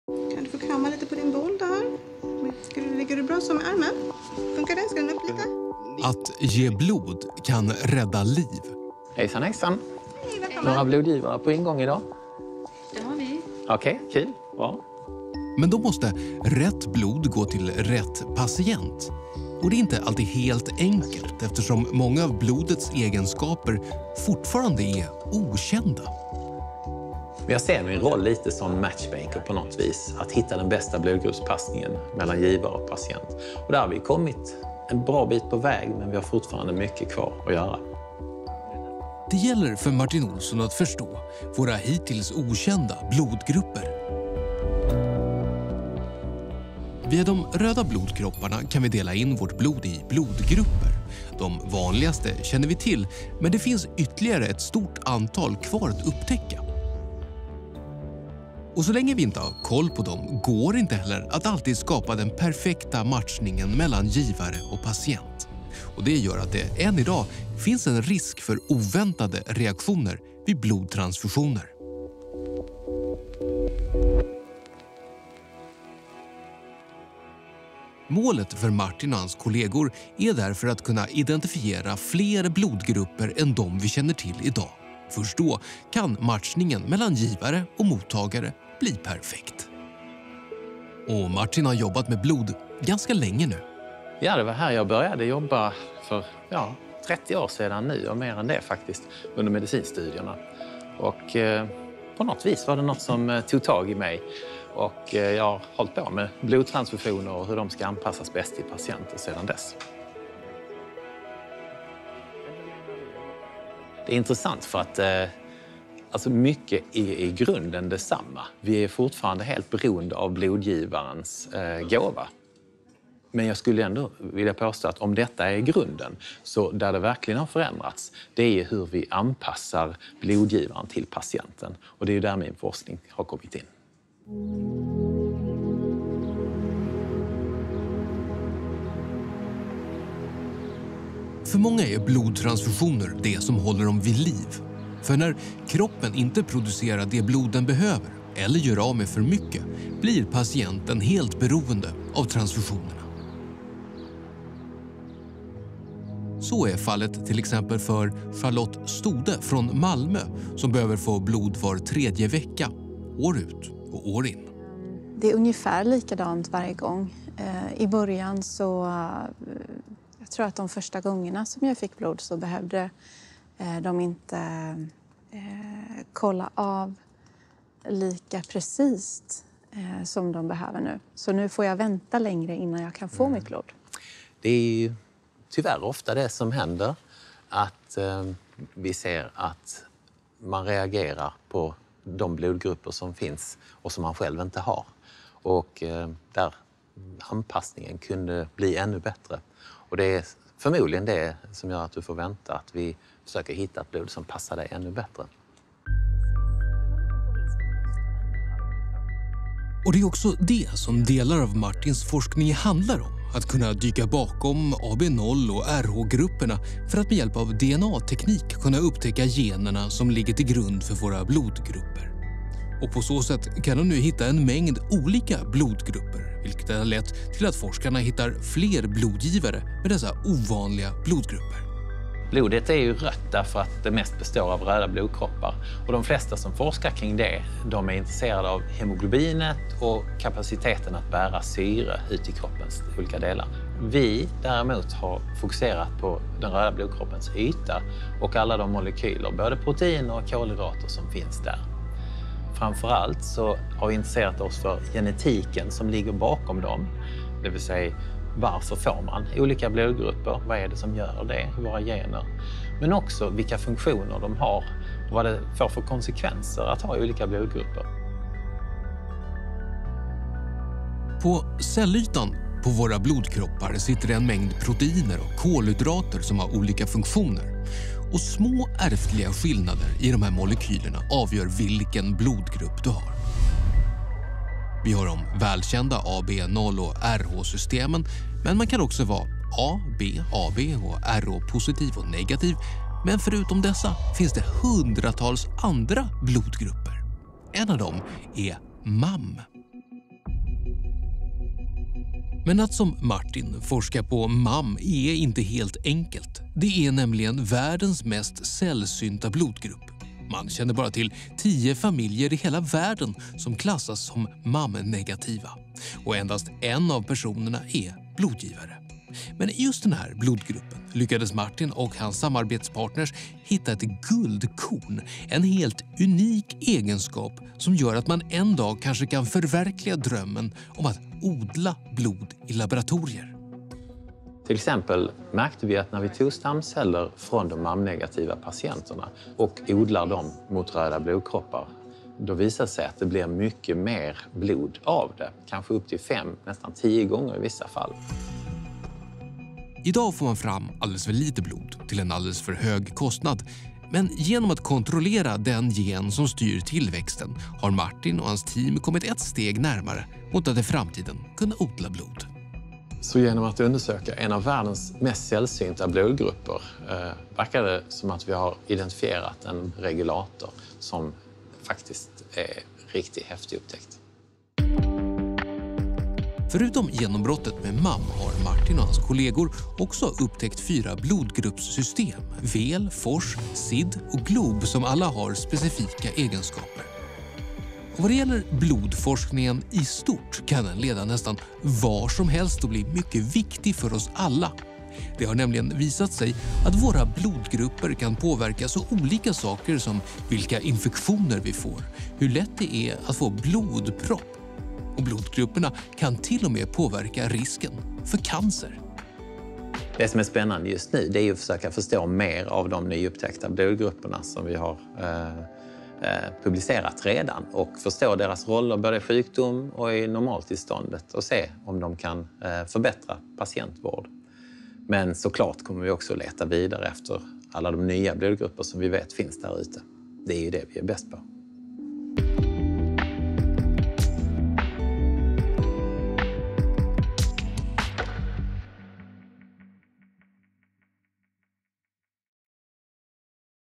Ett första steg mot att odla blod
Wallenberg Clinical Scholar Martin L Olsson och hans kollegor forskar om blodgrupper. Ett mål är att identifiera fler blodgrupper än de vi känner till i dag, för att matchningen mellan blodgivare givare och mottagare ska bli perfekt. Det är något som de också redan lyckats med. När de hittade den mycket sällsynta blodgruppen MAM öppnades också en möjlighet till att i framtiden odla blod i laboratorium.
Martin och hans kollegor vid Lunds universitet har också upptäckt fyra blodgruppssystem som alla har specifika egenskaper. Detta kan vara viktigt för behandling av flera sjukdomar eftersom det visat sig att blodgrupper kan påverka så olika saker som vilka infektioner vi får och hur lätt det är att få blodpropp. De kan till och med påverka risken för cancer.